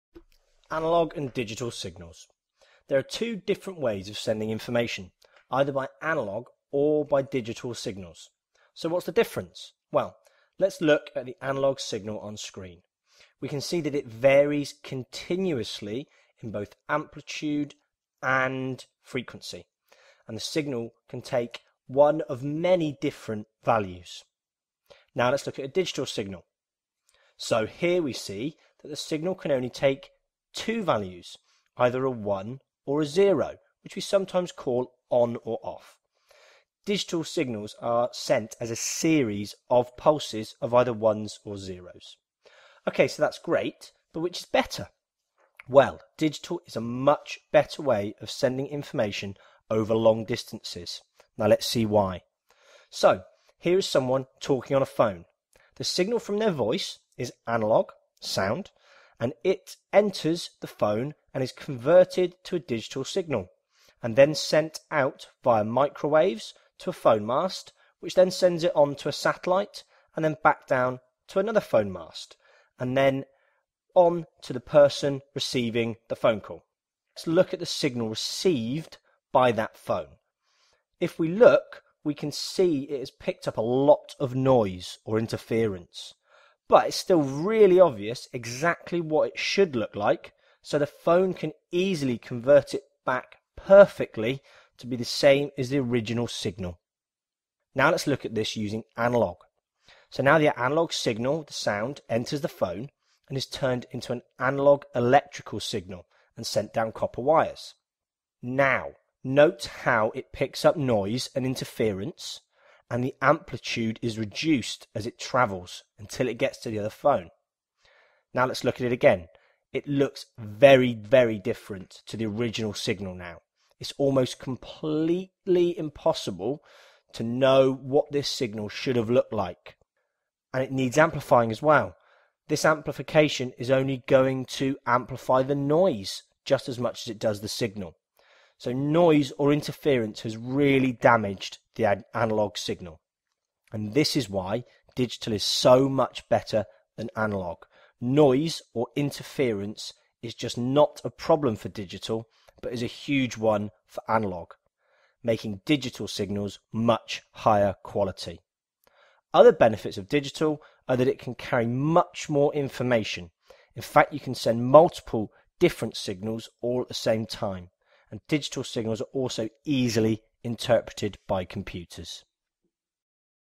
Physics - Waves - Analogue and Digital Signals
A High school science GCSE Physics revision video all about analogue and digital signals. For edexel, AQA and OCR exam boards and iGCSE. Covering interference and noise and advatages and disadvantages.